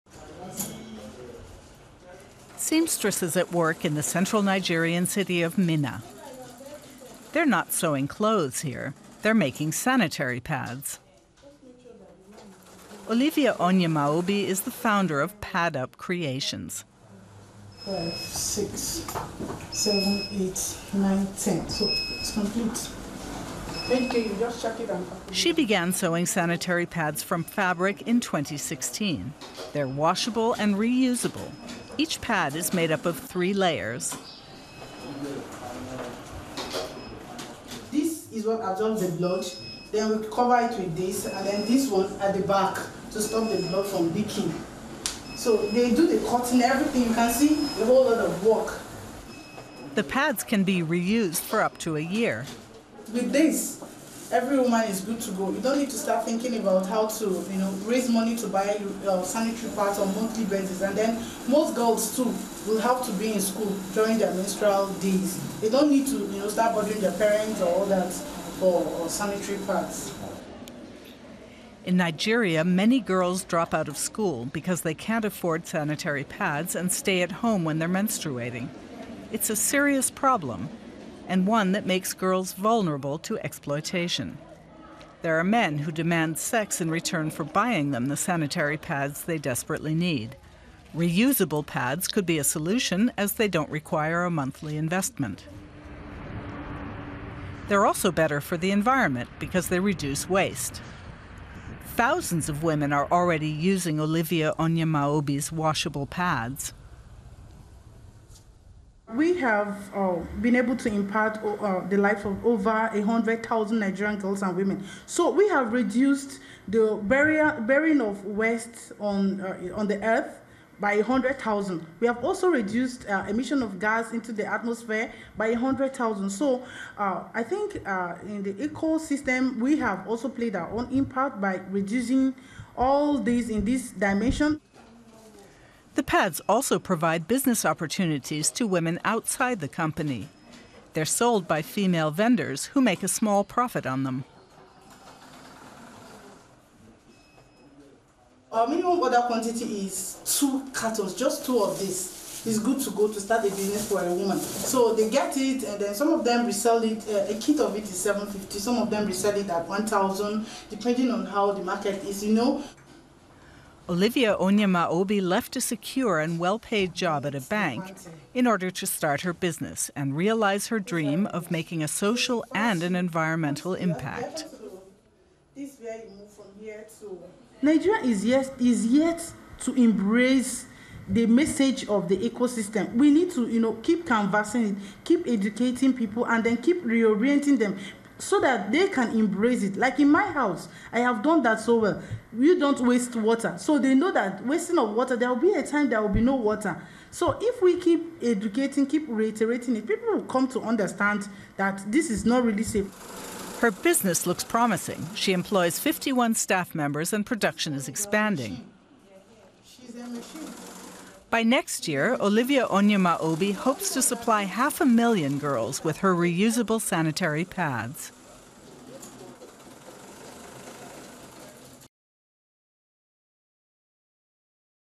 Reusable sanitary pads help Nigerian schoolgirls | DW English
Olivia Onyemaobi founded the company Pad-Up Creations, which produces washable, reusable sanitary pads. This helps the environment — and keeps girls in school.